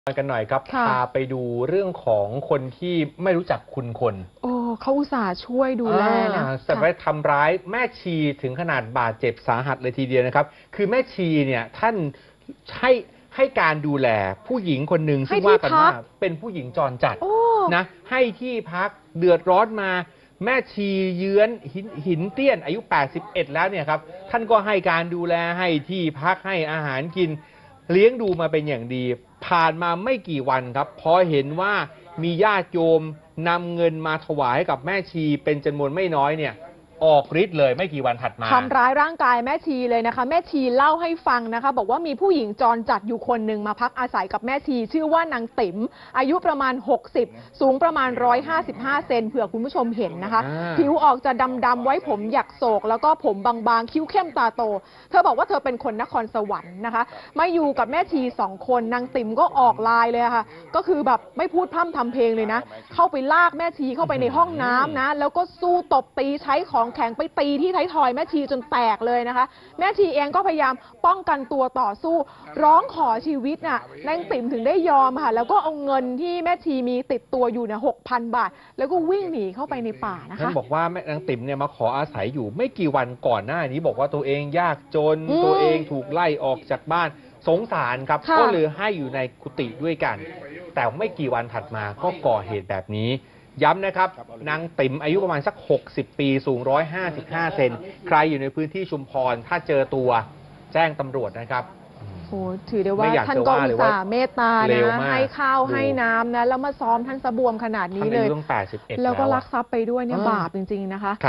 หญิงจรจัดทำร้ายแม่ชีเจ็บสาหัสก่อนฉกเงินหนีลอยนวล
หญิงจรจัดก่อเหตุทำร้ายร่างกายแม่ชีวัย 81 กลางกุฏิ อาการสาหัส ก่อนฉกเงิน 6 พันบาทหนี หลังมาขออาศัยอยู่ด้วยเมื่อหลายวันก่อน คลิก